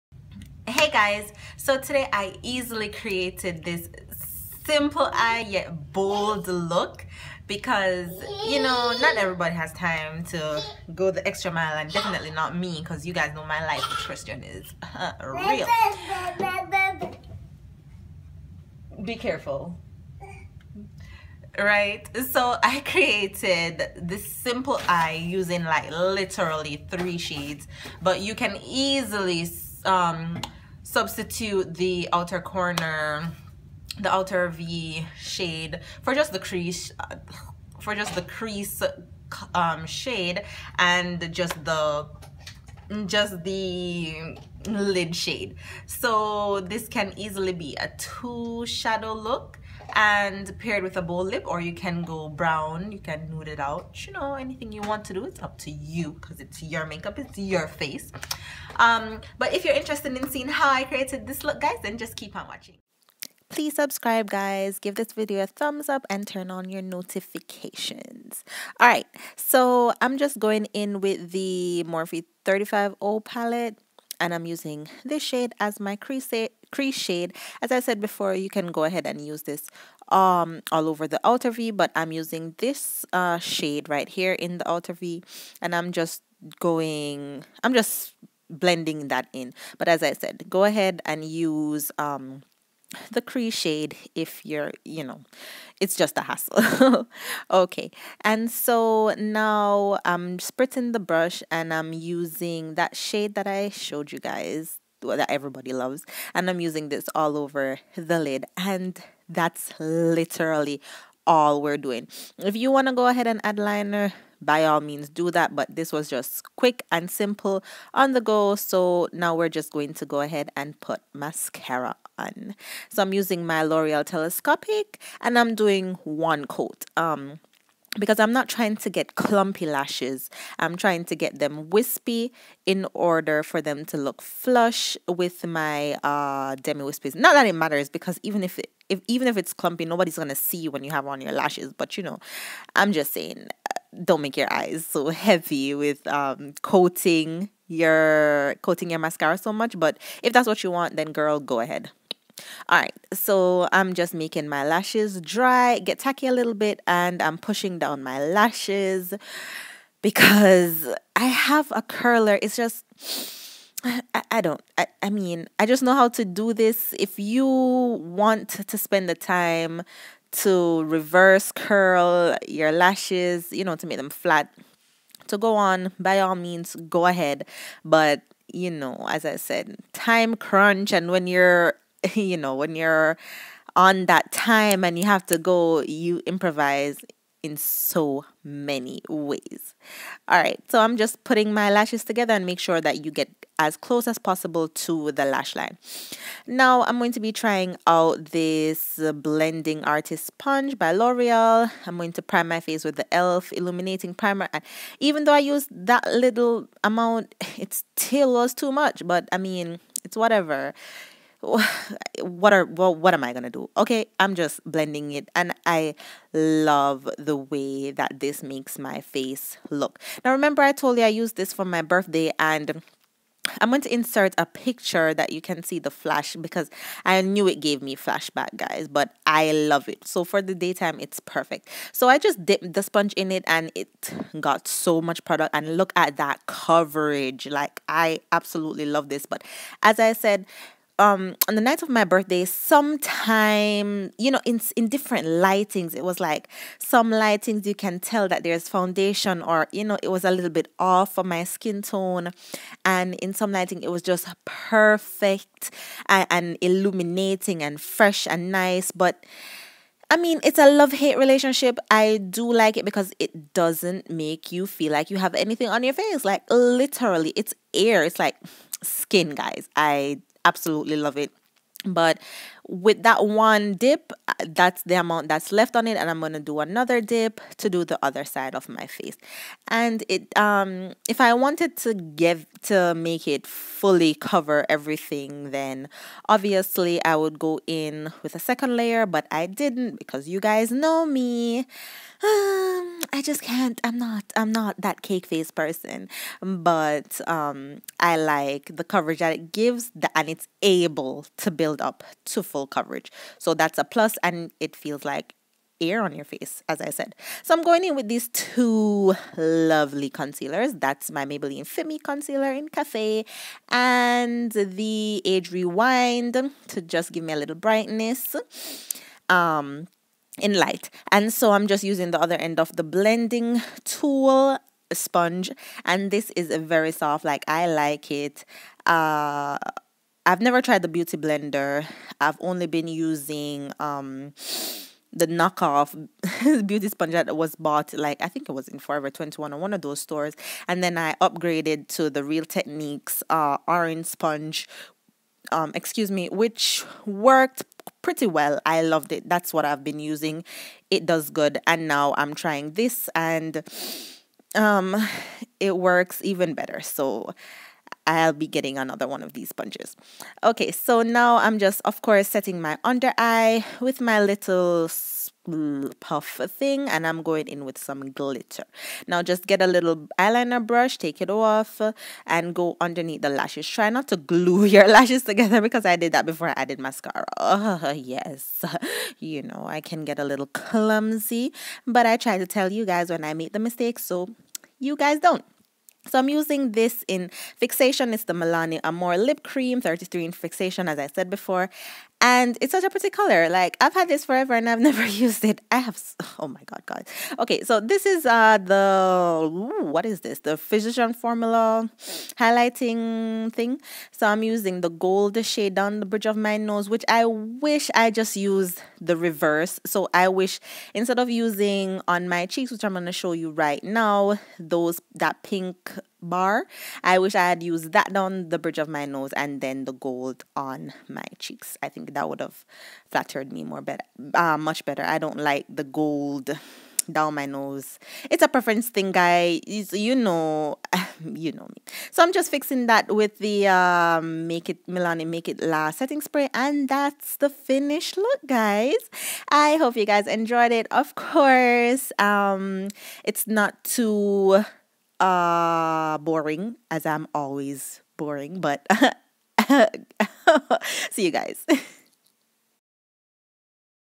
Easy Makeup Tutorial
This is literally the easiest and quickest Makeup Tutorial for a bold statement look!!! Trust me guys, I legit used three shadows and a statement red liquid lipstick but feel free to tone it down using brown or a nude lip! Enjoy 😊

Influenster
All you have to do is sign up, link all your social media sites and start receiving free products to review! 🌟💁🏽

Products Used ❣️

L'Oréal Telescopic Mascara (Black)
Ardell Demi Whispies
Revlon PhotoReady Eye Art (Peach Prism)

ELF Mineral Infused Face Primer (Radiant Glow)
Maybelline Dream Cushion Liquid Foundation (Cocoa)
L'Oréal Infallible Blend Artist Sponge (Generalist)
Maybelline FitMe Concealer (Cafe)
Maybelline Age Rewind Concealer (Light)
L'Oréal Infallible Pro-Matte 16hr Powder (Cocoa)
Physicians Formula Shimmer Strip for Face & Eyes (Warm Nude)
Milani Make It Last Setting Spray (Prime+Correct+Set)

Milani Amore Matte Lip Crème (Fixation)